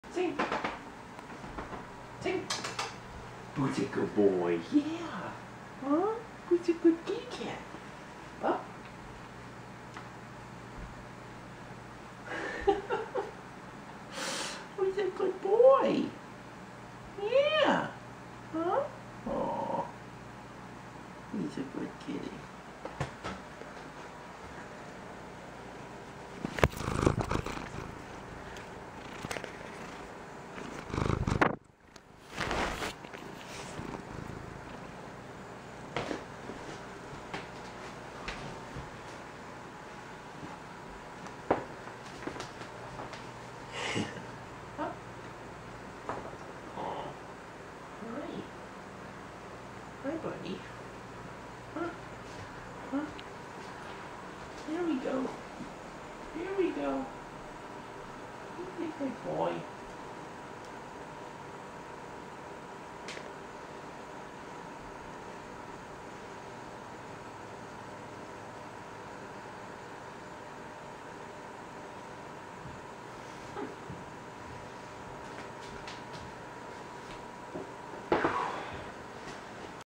Back Cat - Astronocats
Sagan loves hopping on backs! We encourage it and now he does it any time you lean down.